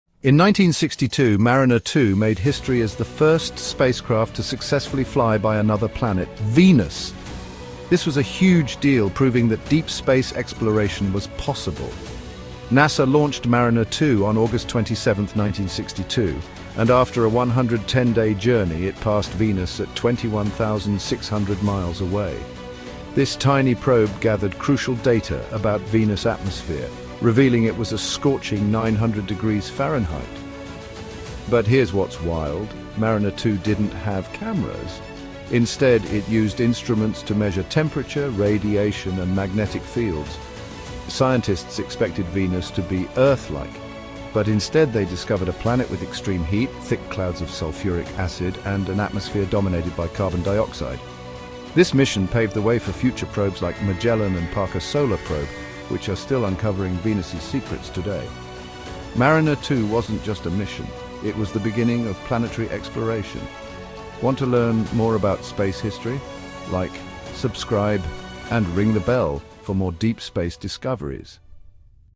Mariner 2's Epic Venus Flyby A Space Milestone!
In 1962, Mariner 2 became the first spacecraft to successfully fly by another planet, Venus! This historic mission paved the way for planetary exploration. 🌌✨

The historic venus flyby by Mariner 2 marked a groundbreaking achievement in the mariner program, showcasing humanity's first successful planetary encounter! 🚀 Using advanced scientific instruments, the probe revealed scorching temperatures of 900°F on Venus's surface, while the space race between nations pushed exploration forward. 🌍🛰️